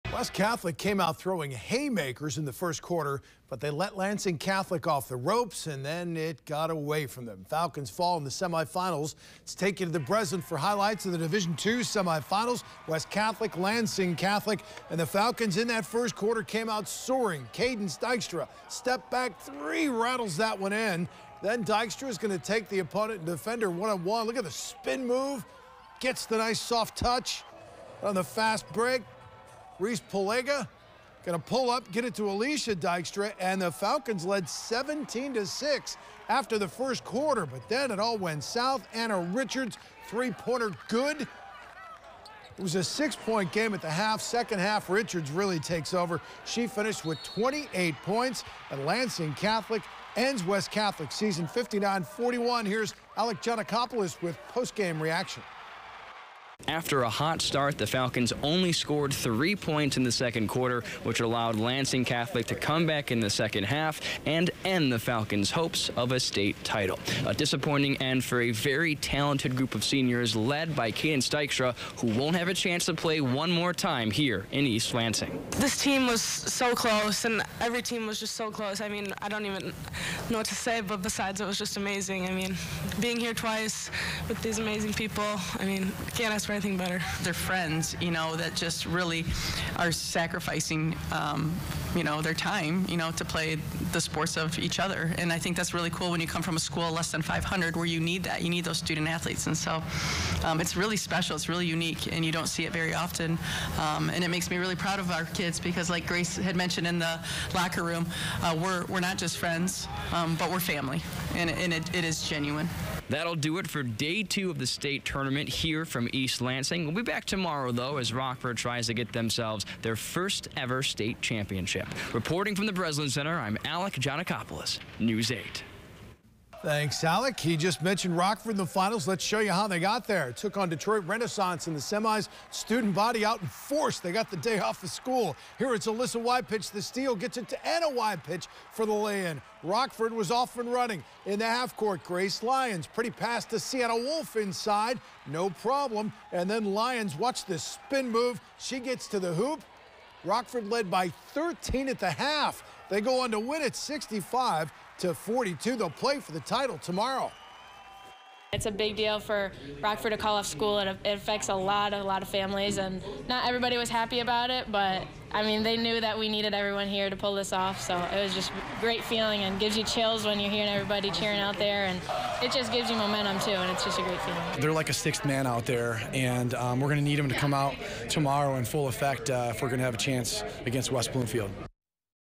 West Catholic vs. Lansing Catholic
West Catholic lost to Lansing Catholic in the semifinals. (March 17, 2023)